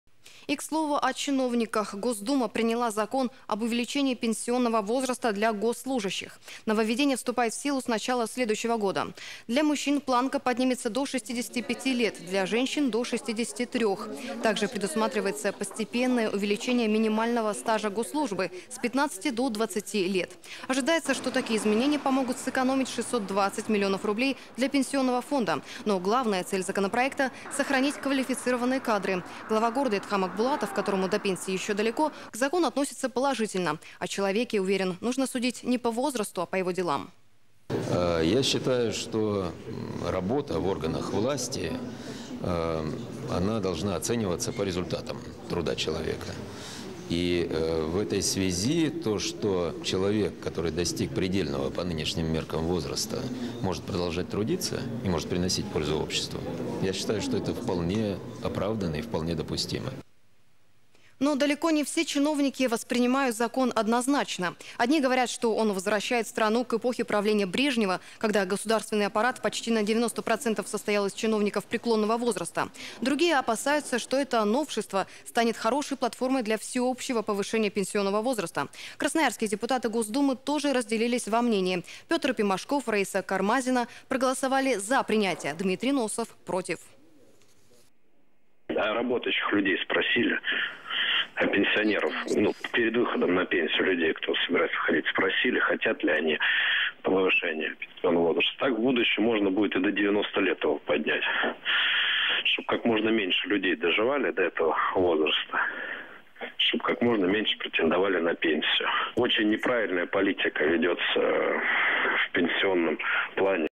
Пенсионный возраст госслужащих увеличился
Для мужчин планка поднимется до 65 лет, для женщин - до 63. Также предусматривается постепенное увеличение минимального стажа госслужбы с 15 до 20 лет